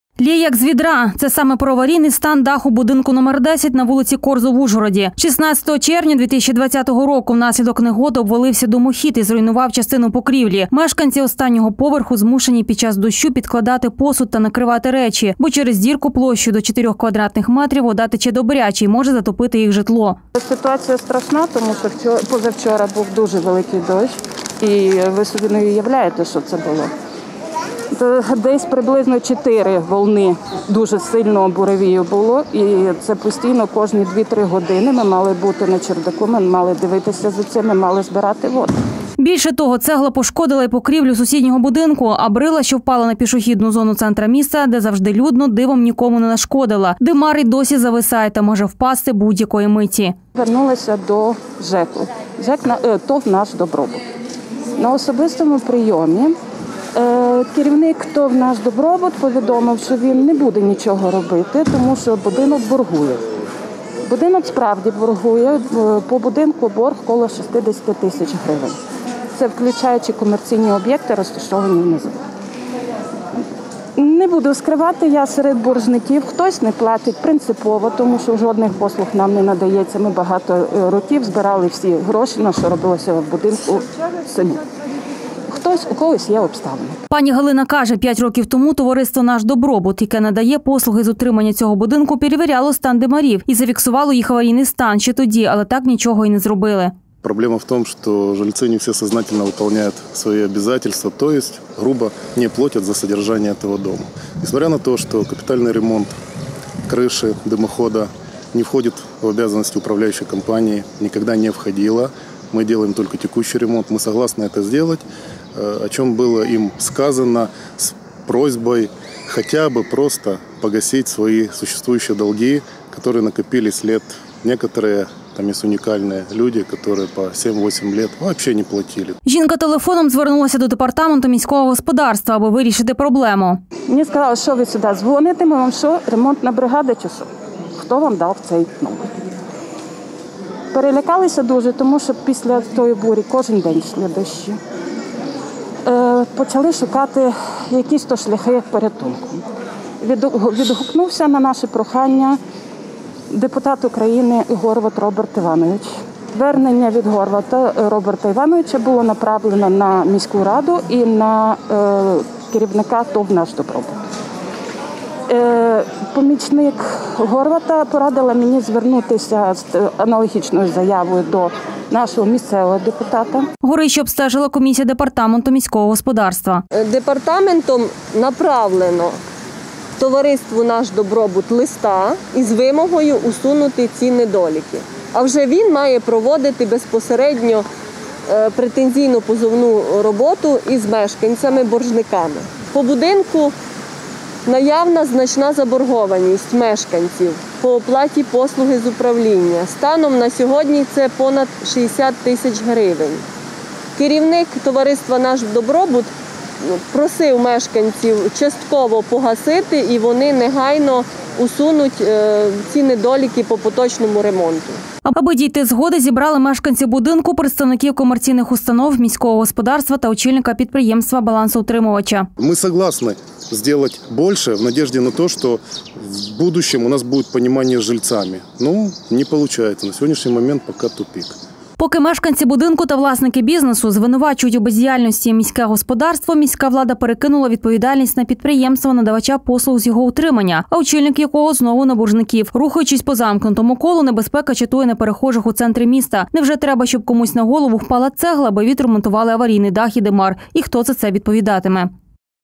Аварійний дах
Корзо в Ужгороді. 16 червня 2020року  внаслідок негоди обвалився димохід і зруйнував частину покрівлі. Мешканці останнього поверху змушені під час дощу  підкладати посуд та накривати речі, бо через дірку площею до 4 квадратних метрів вода тече добряче і може затопити їх житло.
Більше того, цегла пошкодила і покрівлю сусіднього будинку, а брила, що впала на пішохідну зону центра міста, де завжди людно, дивом нікому не нашкодила. Димар і досі звисає та може впасти будь-якої миті.
Пані Галина каже, п’ять років тому ТОВ «Наш Добробут», який надає послуги з утримання цього будинку, перевіряли стан димарів і зафіксували їх аварійний стан ще тоді, але так нічого не зробили.
Жінка телефоном  звернулася до департаменту міського господарства, аби вирішити проблему.
Аби дійти згоди, зібрали мешканців будинку, представників комерційних установ, міського господарства  та очільника підприємства-балансоутримувача.
Поки мешканці будинку та власники бізнесу звинувачують у бездіяльності міське господарство,  міська влада перекинула відповідальність на підприємство- надавача послуг з його у тримання, а очільник якого знову на боржників, рухаючись по замкненому колу, небезпека чатує на перехожих у центрі міста. Невже, треба, щоб комусь на голову впала цегла, аби відремонтувати аварійний димар і дах. І хто за це відповідатиме?